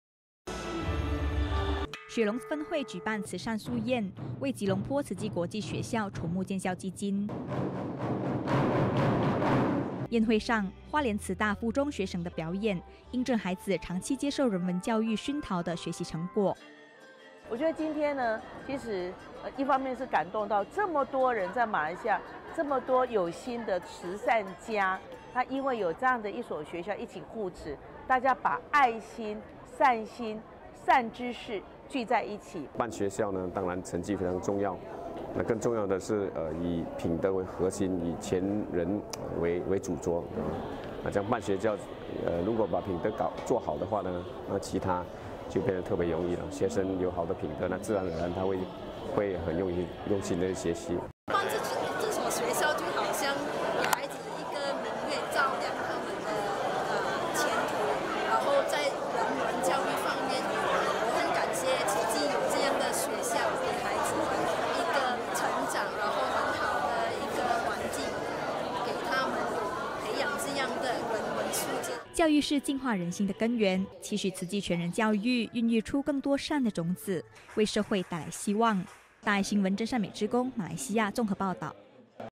雪隆素宴護持慈校 培育全人造就棟梁
為籌建吉隆坡慈濟國際學校建校基金，慈濟雪隆分會舉辦慈善素宴，廣邀社會大眾一起來護持慈濟國際學校，成就孩子的教育殿堂。